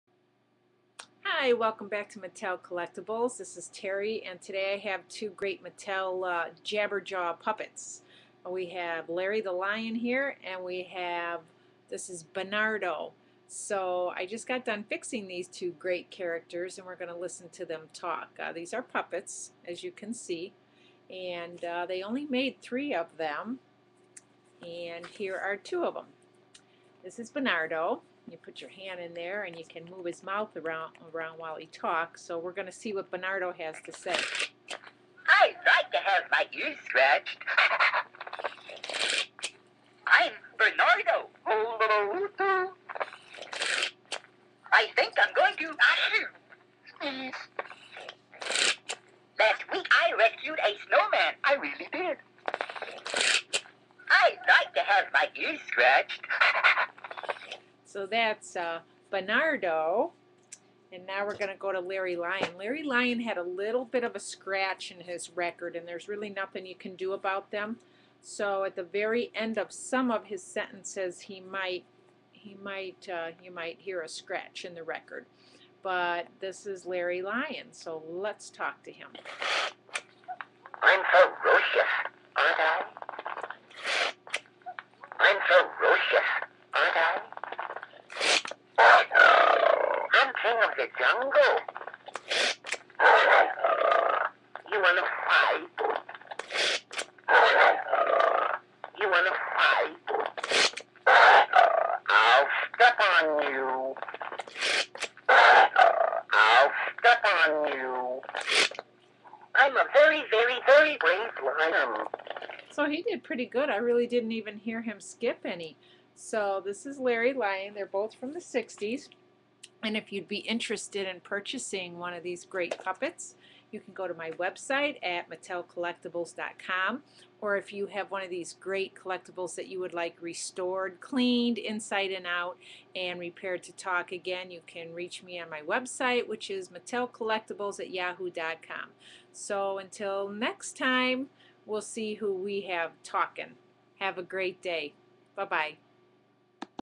mattel talking puppets Bernardo & Larry Lion with Terri
Great jabber jaws puppets from the 60s.  Mattel's Bernardo and Larry the Lion. Excellent vintage condition with tags and talking.